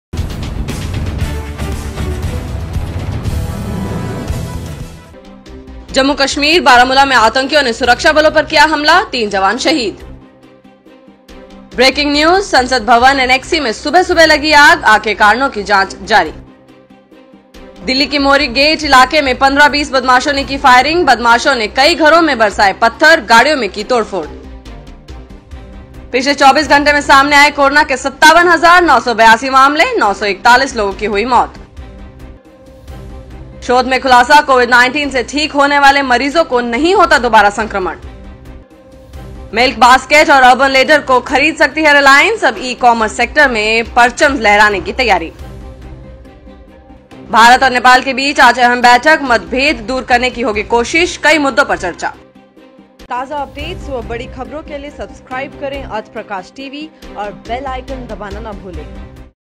ताज़ा हेडलाइंस : शोध में खुलासा Corona पर हुआ बड़ा खुलासा जानें
ताज़ा हेडलाइंस : संसद भवन ऐनेक्सी में सुबह सुबह लगी आग, आग के कारणों की जाँच जारी
जम्मू-कश्मीर बारामुला में आतंकियों ने सुरक्षाबलों पर किया हमला, 3 जवान शहीद
दिल्ली के मोरी गेट इलाके में कौन है बदमाश जिन्होंने मचाया आतंक
शोध में खुलासा कोरोना पर हुआ बड़ा खुलासा जानें
रिलायंस अब ई-कॉमर्स सेक्टर में परचम लहराने की तैयारी में
भारत और नेपाल के बीच आज अहम बैठक, मतभेद दूर करने की होगी कोशिश; कई मुद्दों पर चर्चा